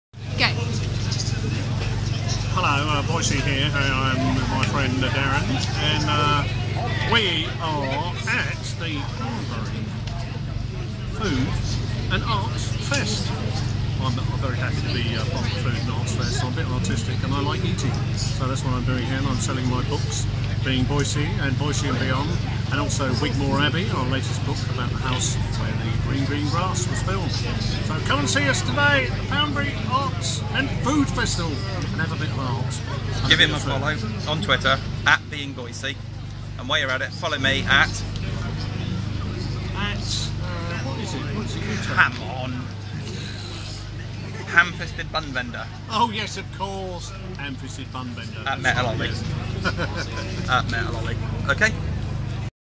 Bun Vendor and Boycie
Hanging out with John Challis and his lovely wife, Carol, at the Dorset Food and Arts Festival.